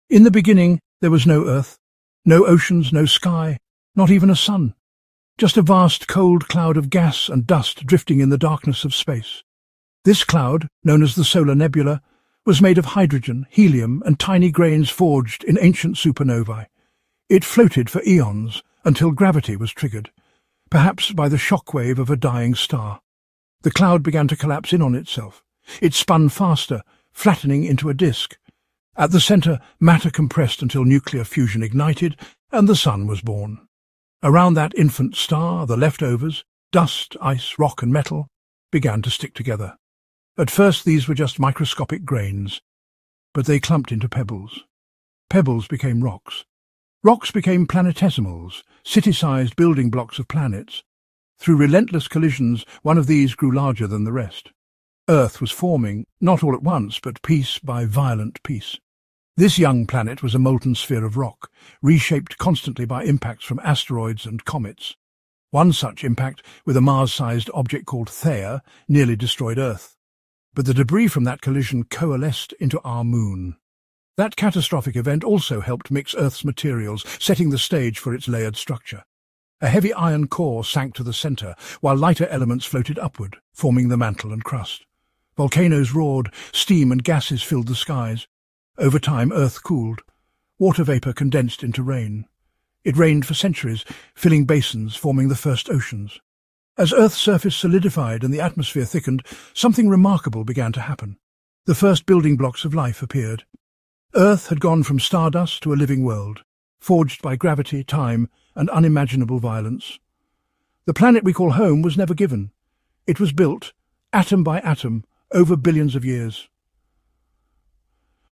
How Was The Earth Formed?
Discover the epic journey of our planet — from cosmic dust to the vibrant world we call home 🌍✨

Dive into the explosive events, fiery collisions, and cosmic miracles that built Earth atom by atom over billions of years. This is the story of life’s ultimate origin, told like never before.